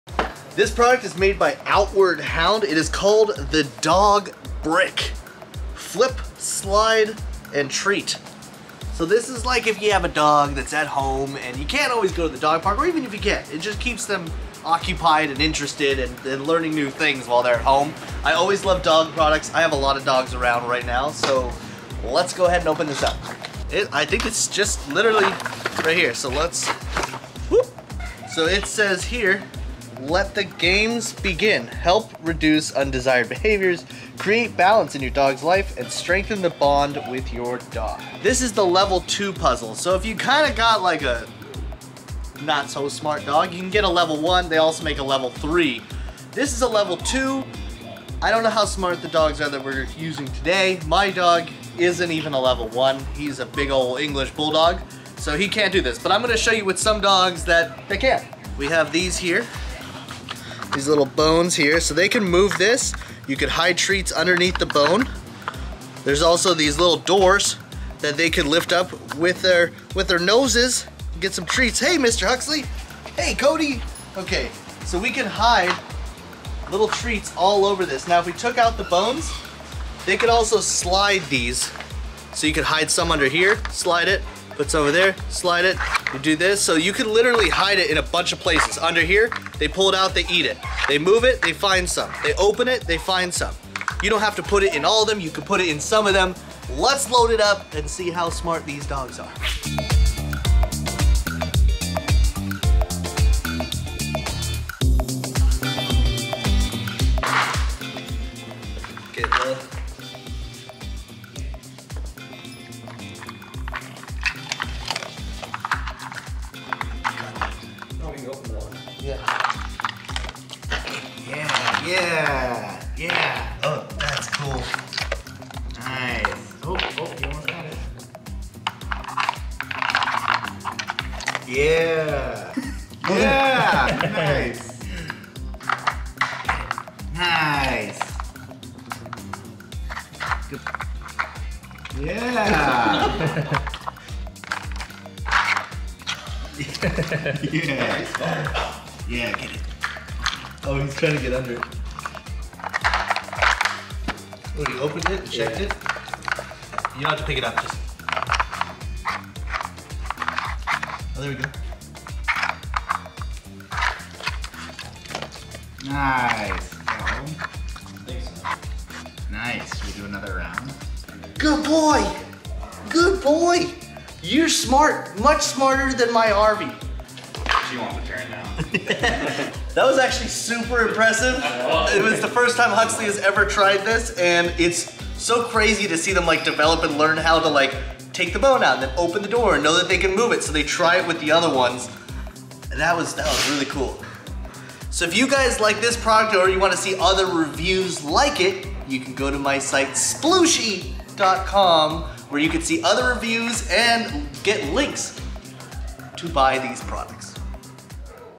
Interactive Dog Puzzle Game Review
I was actually shocked how quickly a dog will begin to problem solve to find the hidden treats. This was a lot of fun to watch and play with. I highly recommend it for you dog owners!